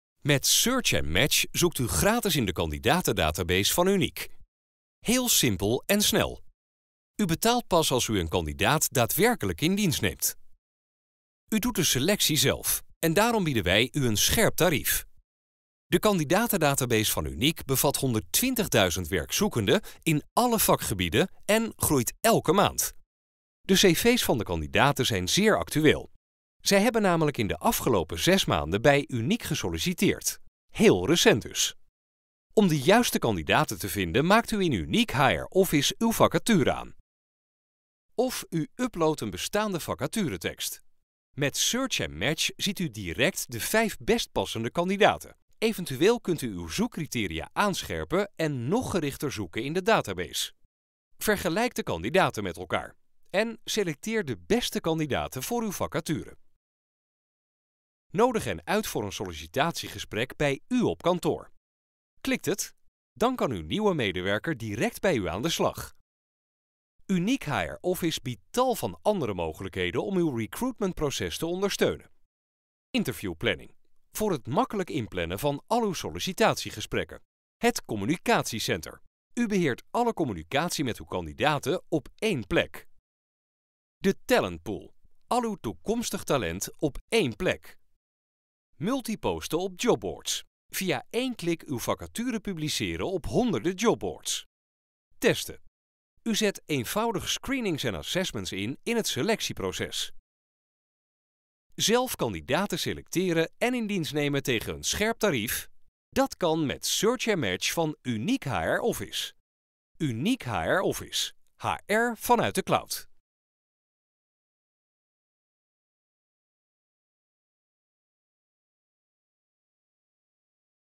Unique Search & Match
U zoekt simpel, snel en gratis in de kandidatendatabase van Unique. Onze database bevat 120.000 werkzoekenden in alle vakgebieden en groeit elke maand. De CV's van de kandidaten zijn zeer actueel.